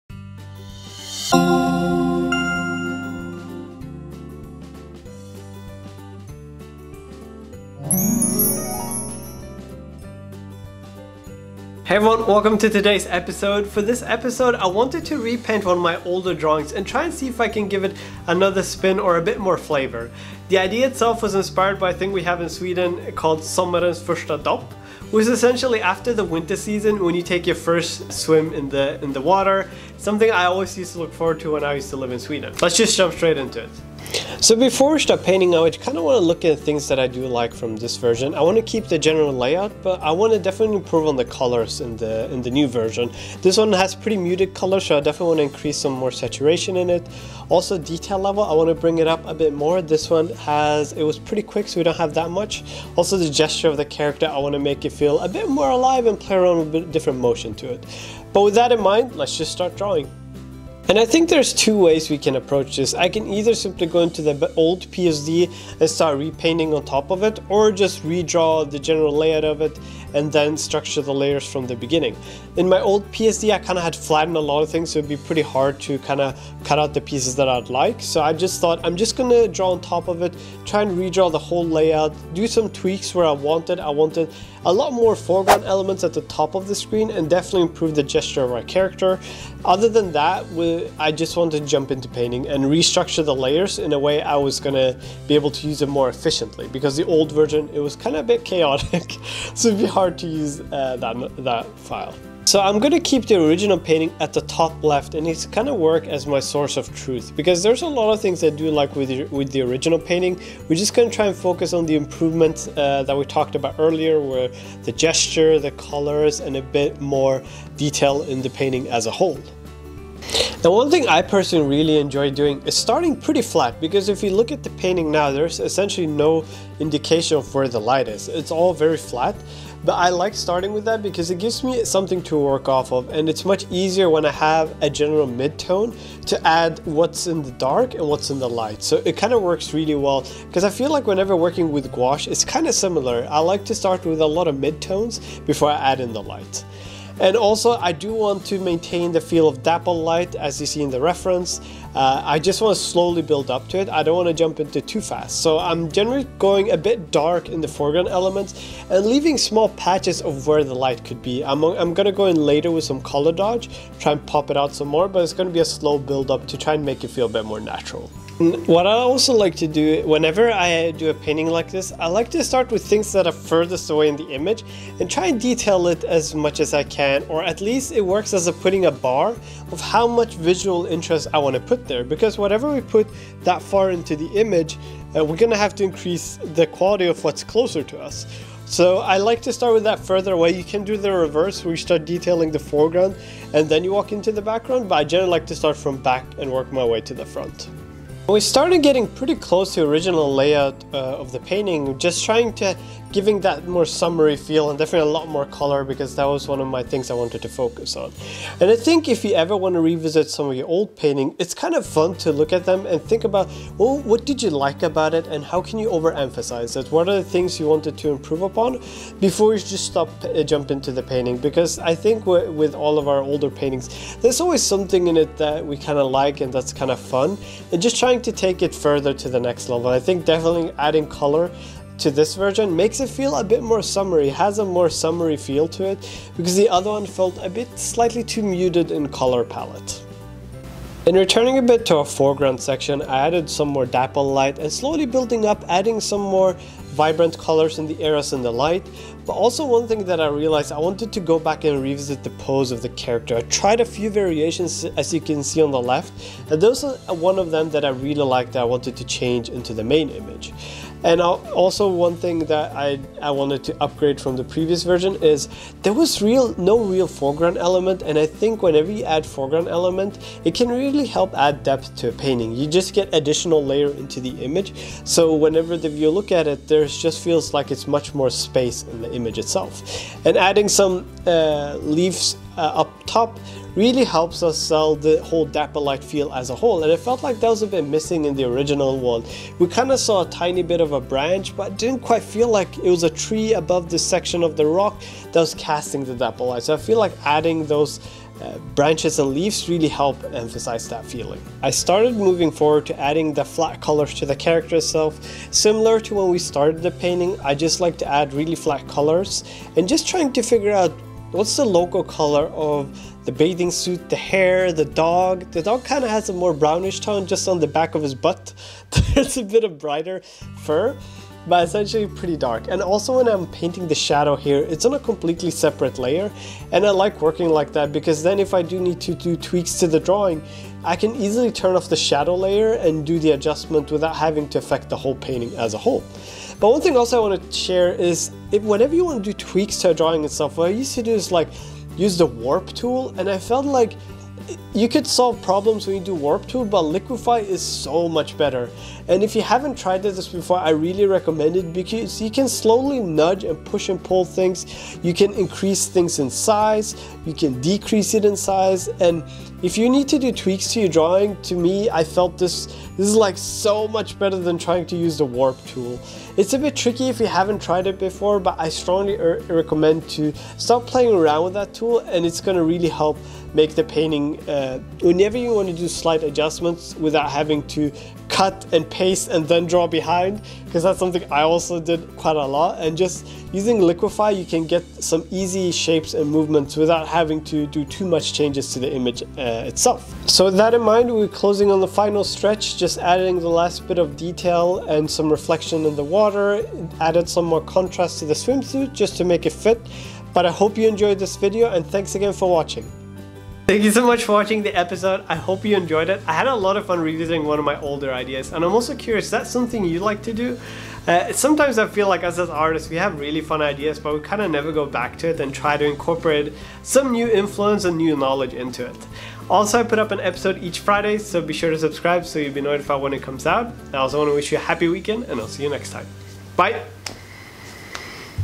Redrawing old art
Hey everyone,

For this week's episode I thought it would be really fun to revisit one of my old paintings. I've seen a lot of other artists do it and It's always really fun to see how you can re-interpret your own image, so I wanted to try as well.

Thanks and have a wonderful weekend!